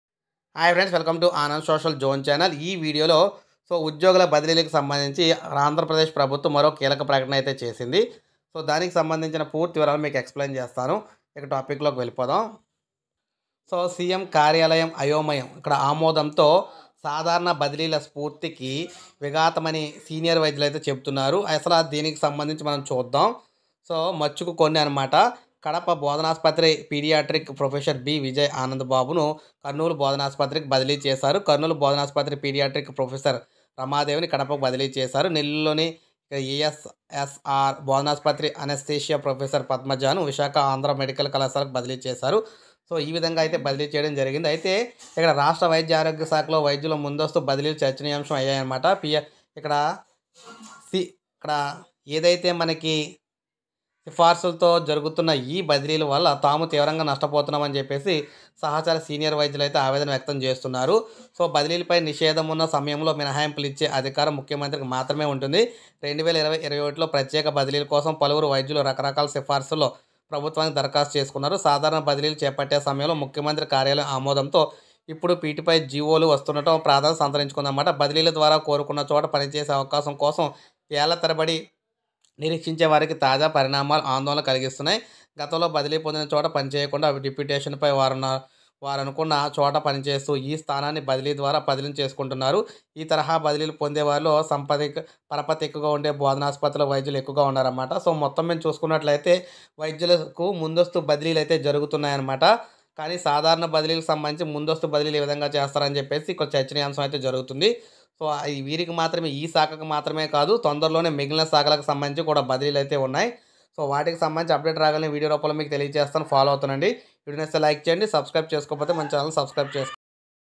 ఉద్యోగుల బదిలీలపై సడన్ ట్విస్ట్ ఇచ్చిన జగన్ ap employees transfer latest update.
hello everyone today update is AP government latest update on employees transfers latest news .@Anandsocialzone